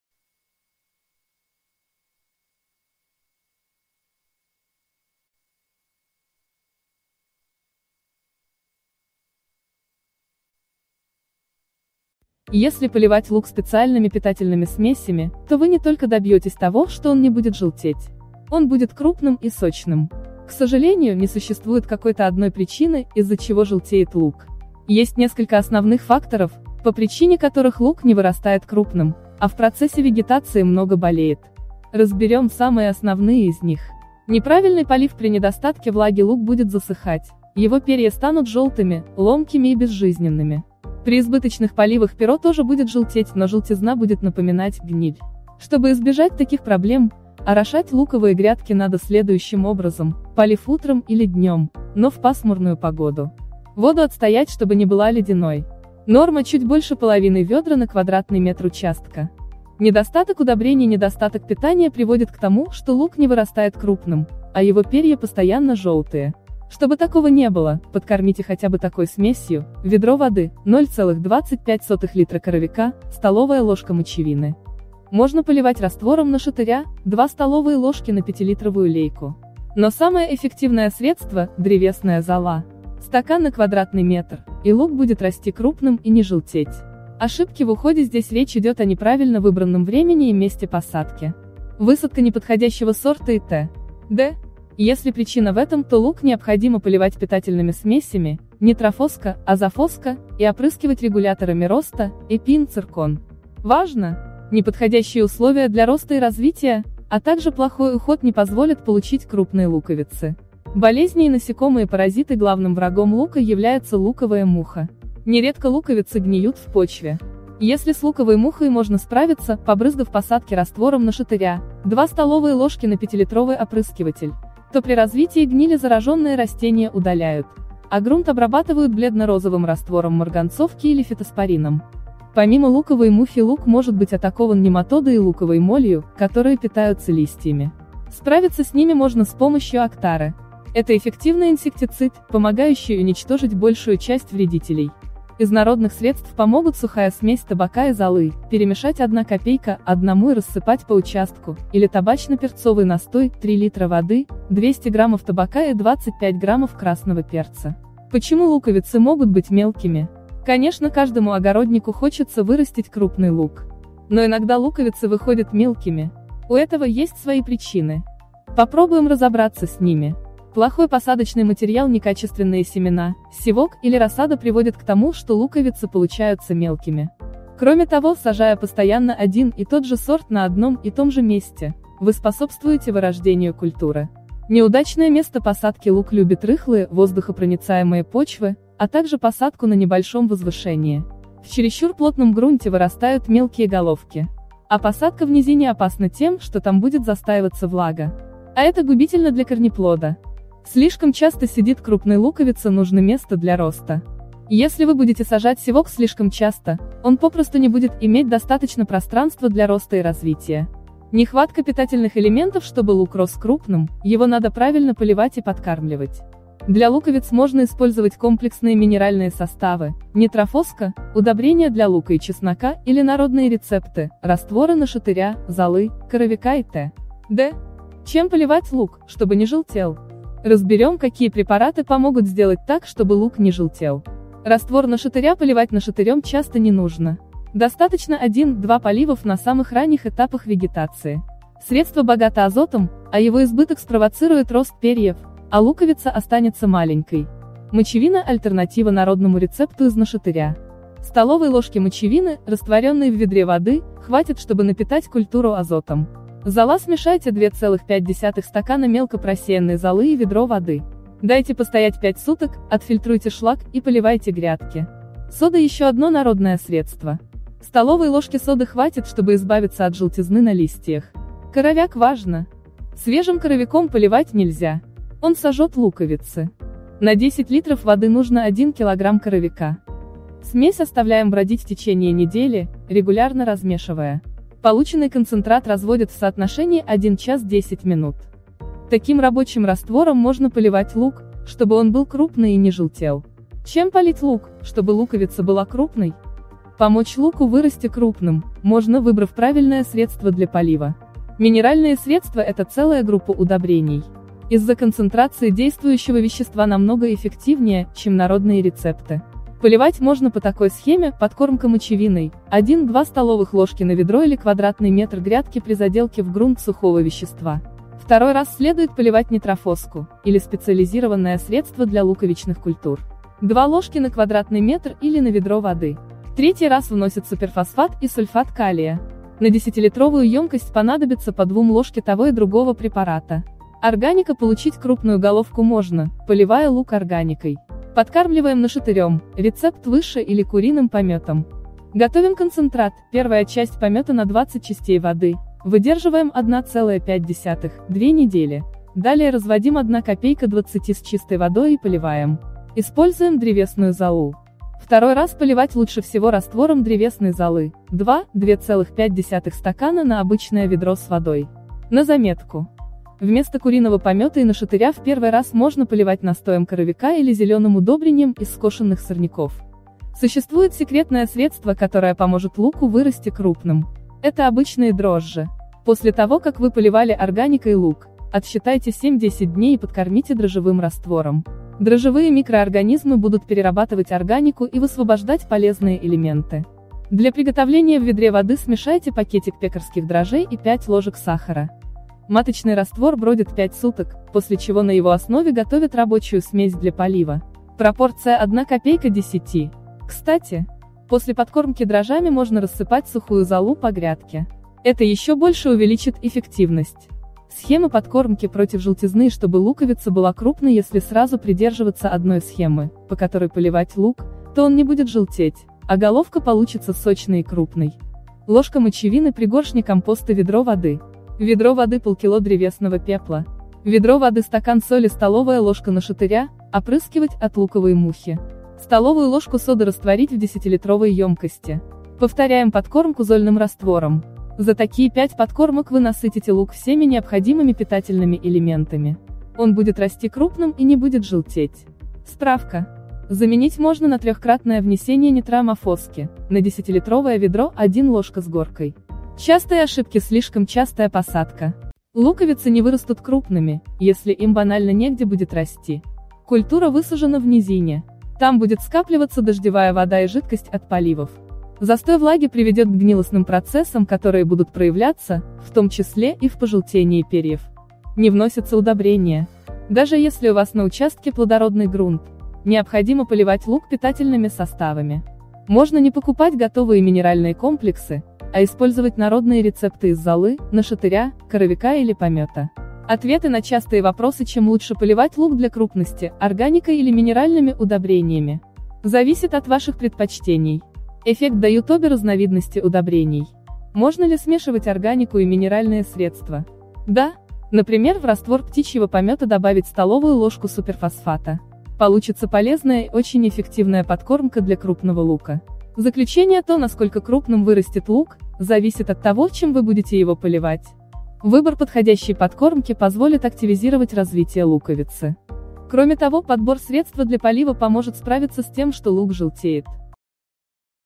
Чем поливать лук чтобы был крупный и не желтел, какие подкормки использовать
Если поливать лук специальными питательными смесями, то вы не только добьетесь того, что он не будет желтеть. Он будет крупным и сочным.